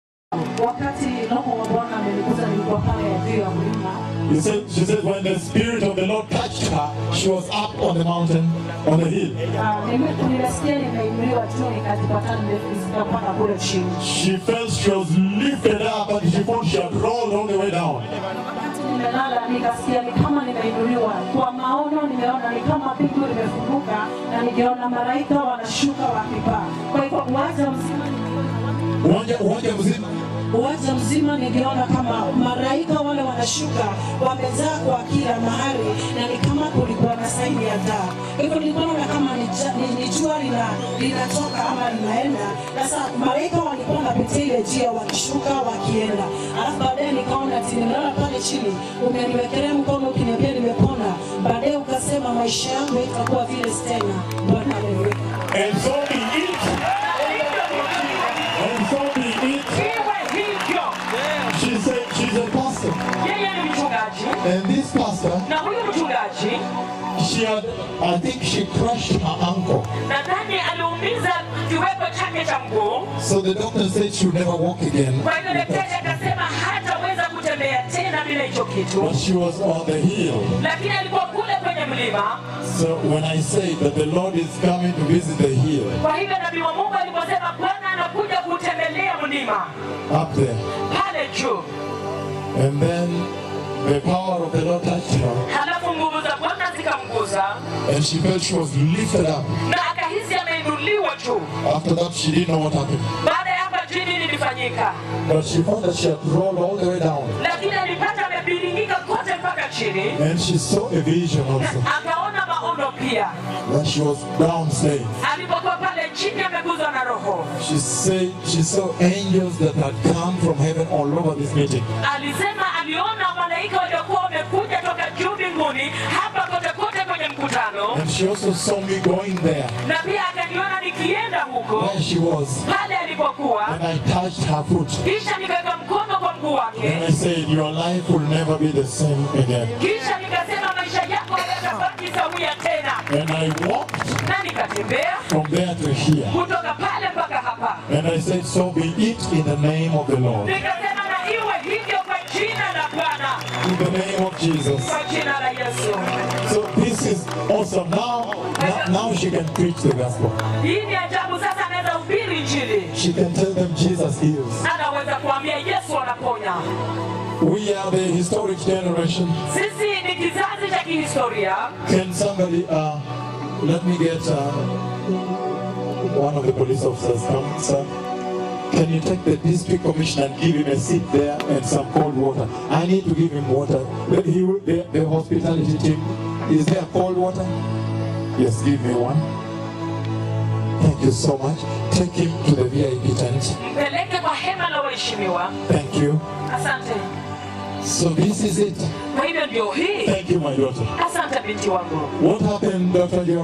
Healing Service In Kericho June 4, 2011 Part 21.mp4
The Repentance Revival that took on June 4, 2011 in Kericho, Kenya. Four million attended this historic event.